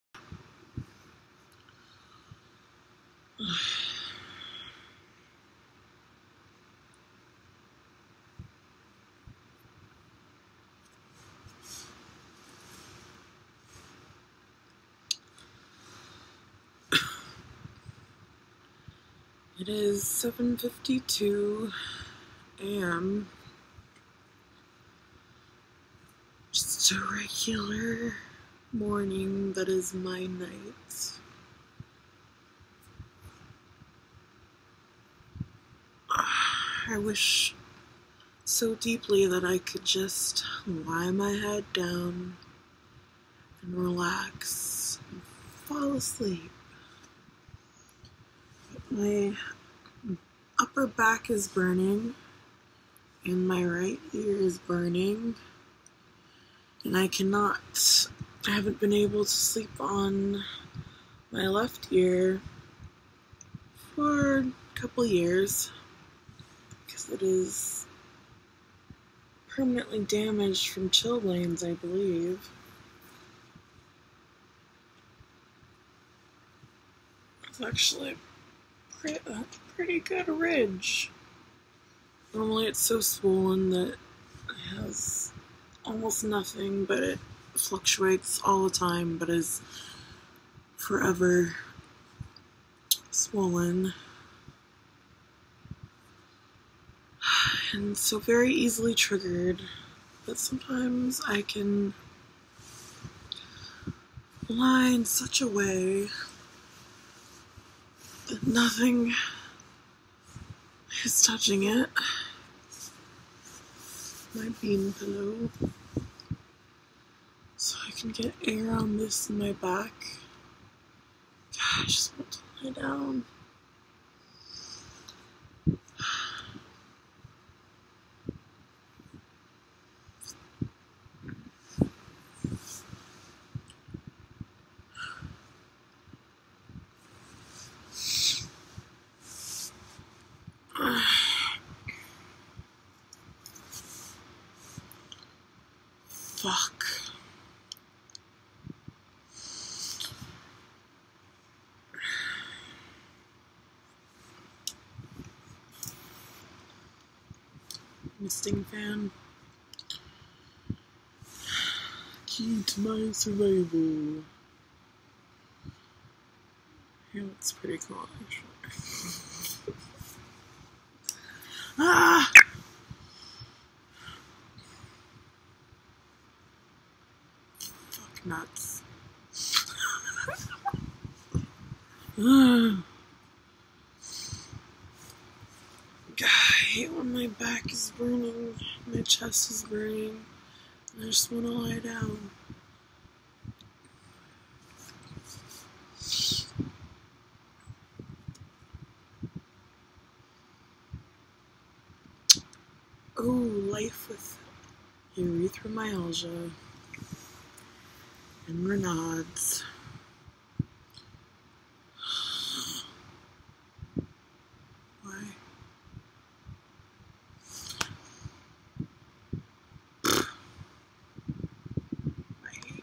Life with EM Part 1
Can't lie down and relax. Can't sleep. The usual. The usual hell.

Can't stop swearing. Sorry not sorry.

Movie on 3 2 16 at 7 51 AM #2